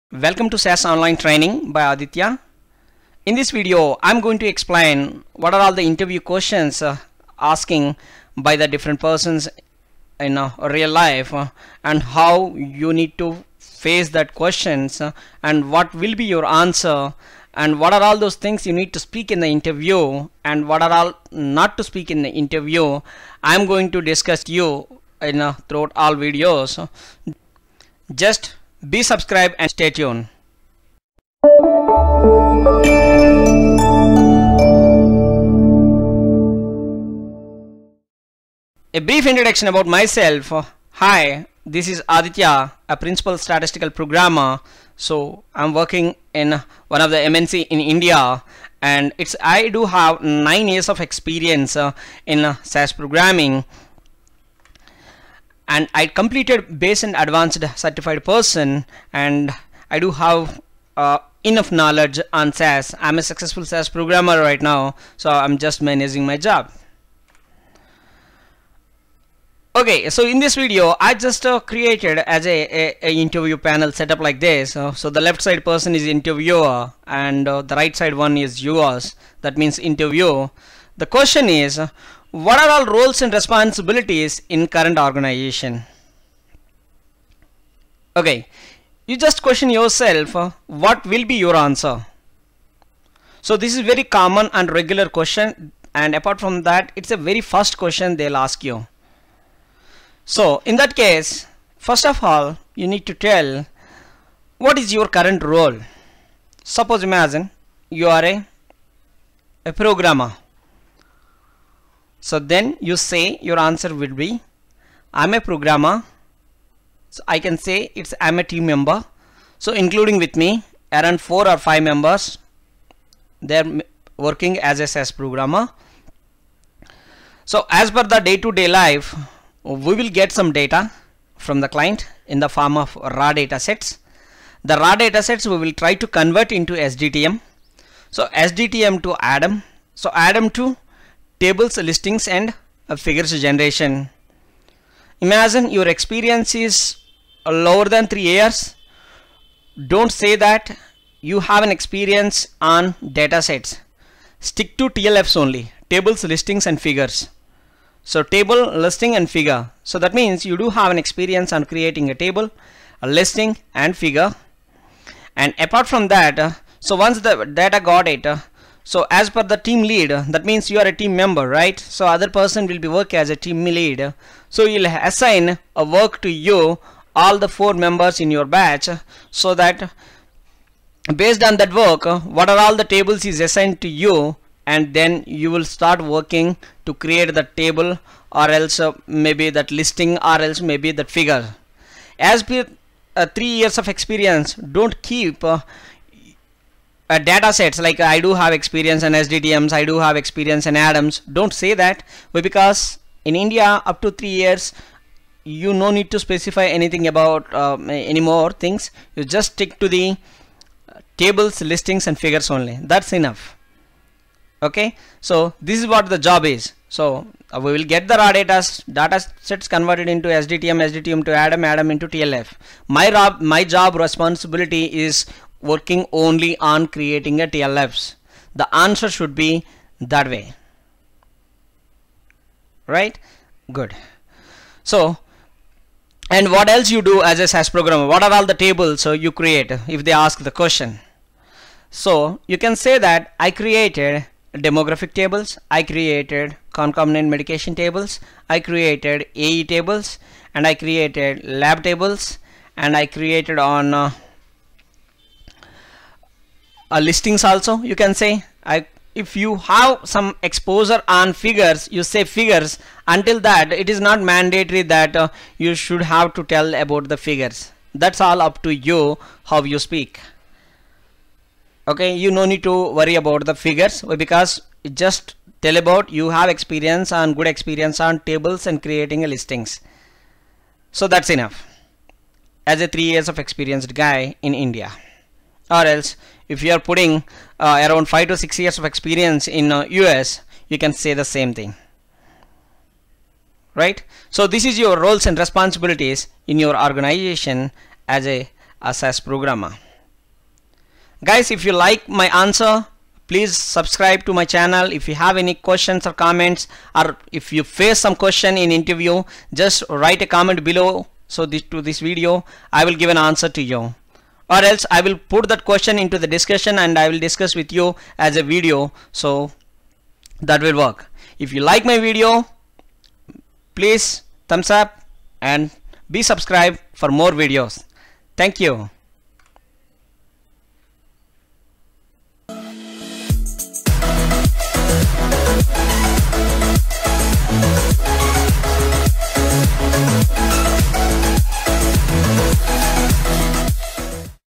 SAS Clinical Interview QUESTIONS By ADITYA Naidu : QUESTION #1
SAS Clinical Interview QUESTIONS and ANSWERS By ADITYA  QUESTION #1. This is about What are your responsibilities as clinical SAS Programmer